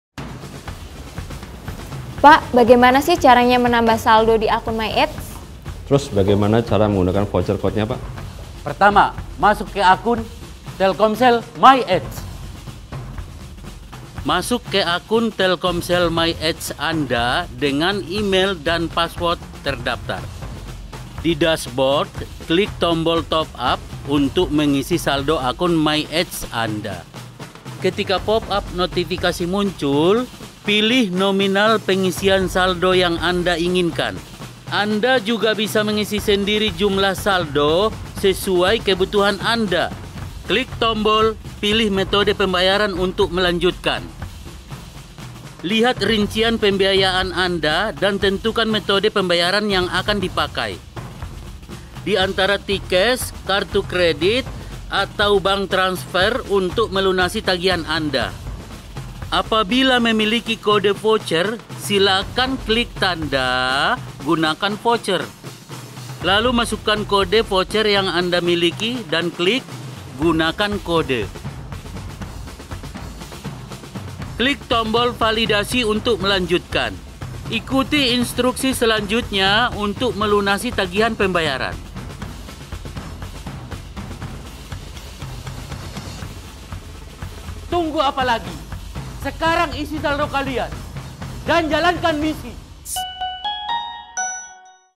Bagaimana Cara To Up Saldo MyAds Telkomsel | Kode Voucher bonus Saldo Top up 100%
Bagaimana Cara To Up Saldo MyAds Telkomsel | Kode Voucher 250ribu Gratis | Digital Marketing

Dalam Video ini dijelaskan bagaimana cara top up saldo Myads telkomsel dan bagaimana cara memasukkan voucher 250ribu gratis buat anda..
selamat mencoba

gunakan kupon AJKINE untuk top up diskon voucher bonus Saldo Top up 25%

~ Terimakasih